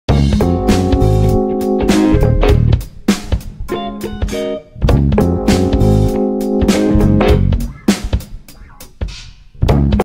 Telephone ☎📞
On March 7, 1876, 29-year-old Alexander Graham Bell receives a patent for his revolutionary new invention: the telephone. The Scottish-born Bell worked in London with his father, Melville Bell, who developed Visible Speech, a written system used to teach speaking to the deaf.